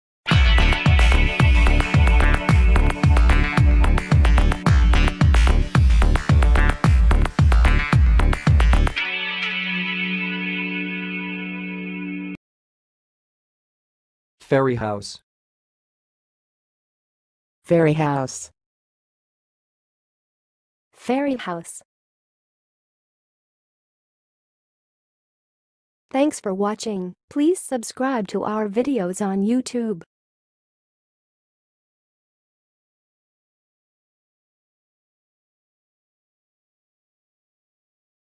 How to say "ferryhouse"! (High Quality Voices)
Watch in this video how to say and pronounce "ferryhouse"!